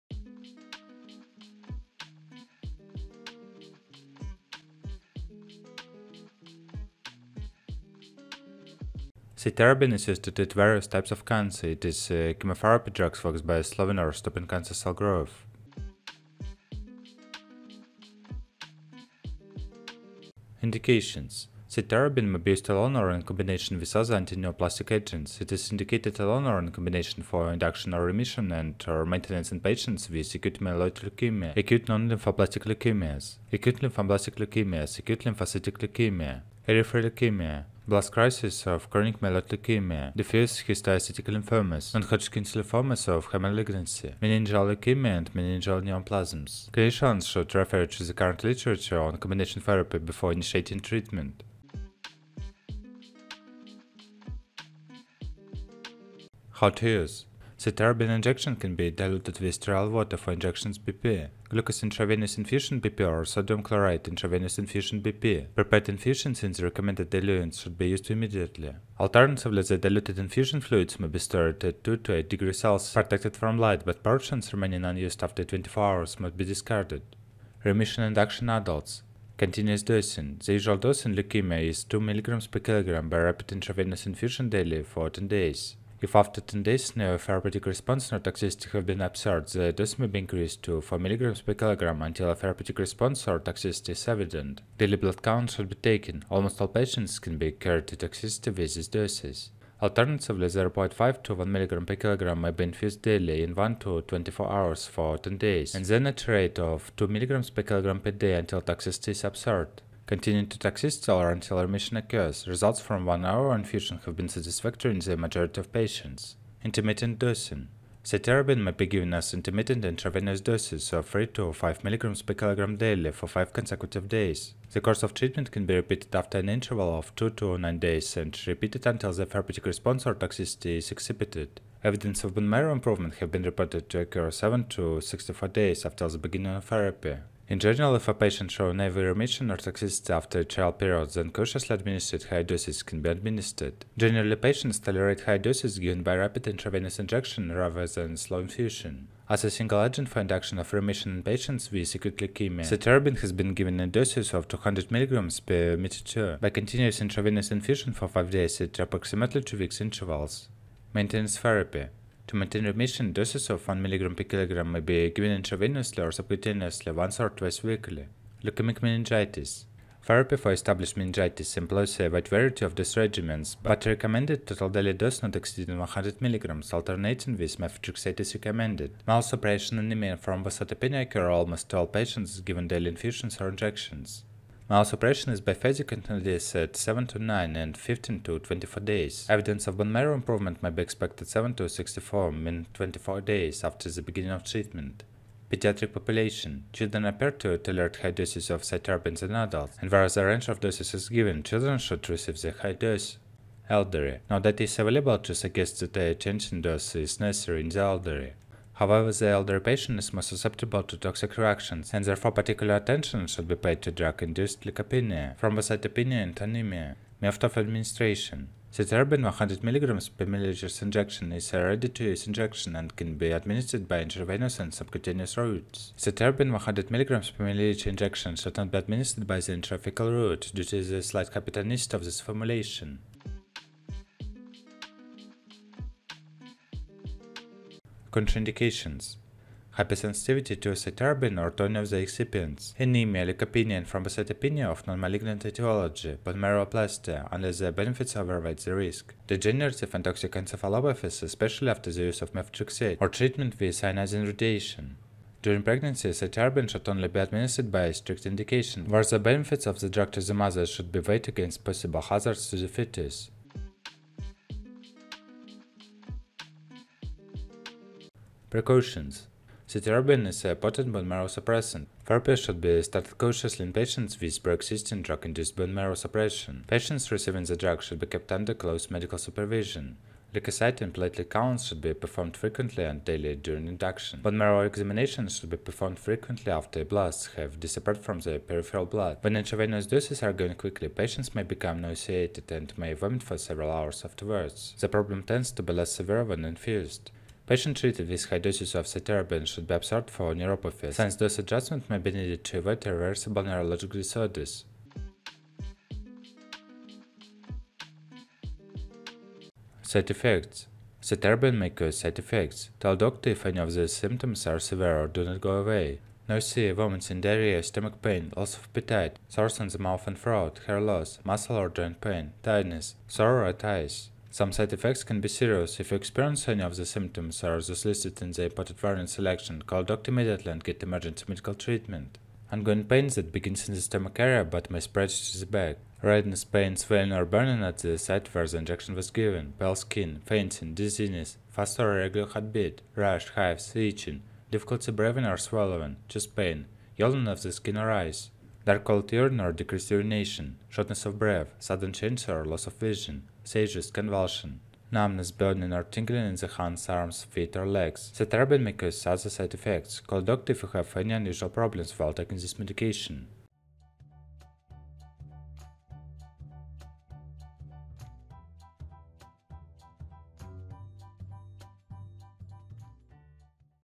Cytarabine injection how to use: Uses, Dosage, Side Effects, Contraindications
Cytarabine is used to treat various types of cancer. It is a chemotherapy drug that works by slowing or stopping cancer cell growth.

Video Content:
0:00 Cytarabine injection
0:15 Indications
0:51 How to use
3:53 Contraindications
4:23 Precautions
5:04 Side effects